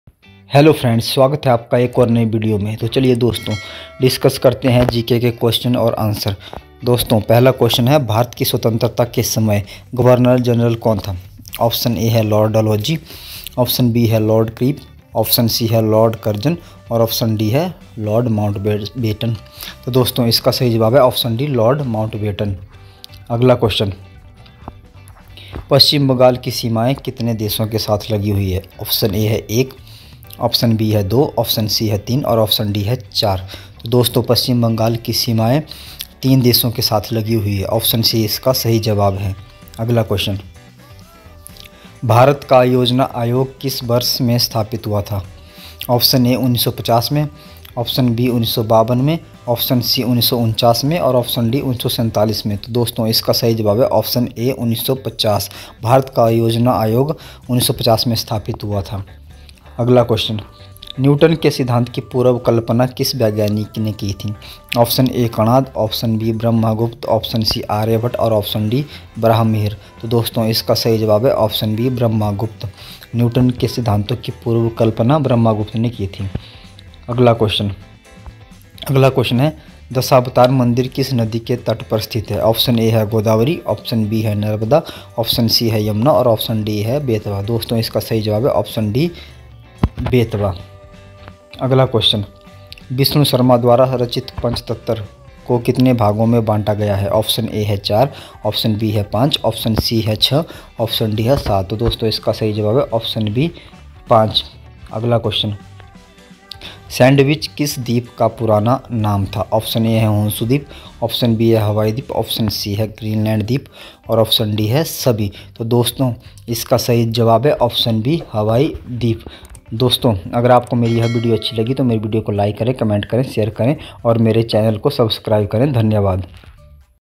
General Awareness Questions in Hindi || सामान्य जागरूकता प्रश्न हिंदी में || GK in Hindi || Part - 4
General Awareness Questions in Hindi || सामान्य जागरूकता प्रश्न हिंदी में || GK in Hindi

सैंडविच किस दीप का पुराना नाम था ?

भारत की स्वतन्त्रता के समय गवर्नल जनरल कौन था ?

पश्चिम बंगाल की सीमाएं कितने देशों के साथ लगी हुई है ?

भारत का योजना आयोग किस वर्ष में स्थापित हुआ था ?

न्यूटन के सिद्धांतो की पूर्व कल्पना किस वैज्ञानिक ने की ?

दशावतार मंदिर किस नदी के तट पर स्थित है ?

विष्णुशर्मा द्वारा रचित पंचतंत्र को कितने भागों में बांटा गया है ?